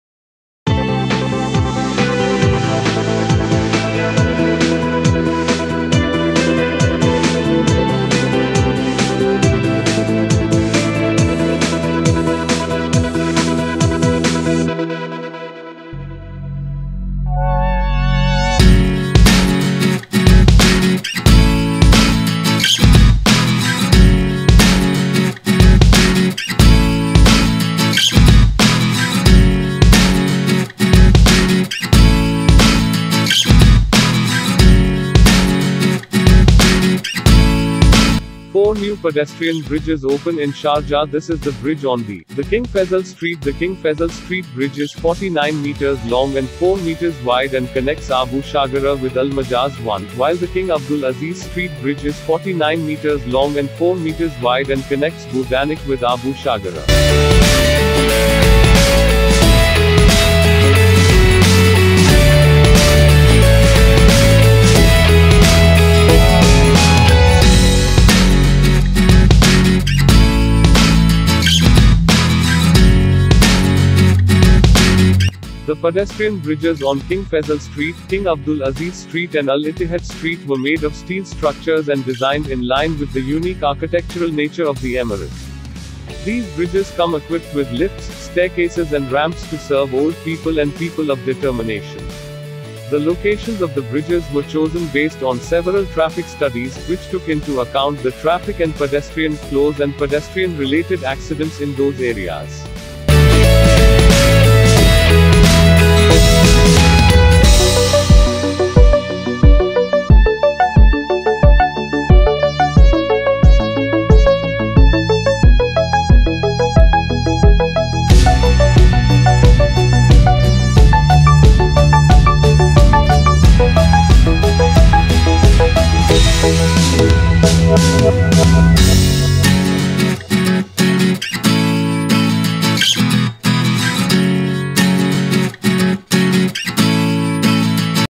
Sharjah, UAE | 4 new pedestrian bridges open in Sharjah | This is on The King Faisal Street
Four new pedestrian bridges open in Sharjah

This is the bridge on The King Faisal Street

The King Faisal Street bridge is 49 metres long and four metres wide and connects Abu Shagara with Al Majaz 1, while the King Abdul Aziz Street bridge is 49 metres long and four metres wide and connects Bu Daniq with Abu Shagara.
Sharjah

The pedestrian bridges on King Faisal Street, King Abdul Aziz Street and Al Ittihad Street were made of steel structures and designed in line with the unique architectural nature of the emirate. These bridges come equipped with lifts, staircases and ramps to serve old people and people of determination.

The locations of the bridges were chosen based on several traffic studies, which took into account the traffic and pedestrian flows and pedestrian-related accidents in those areas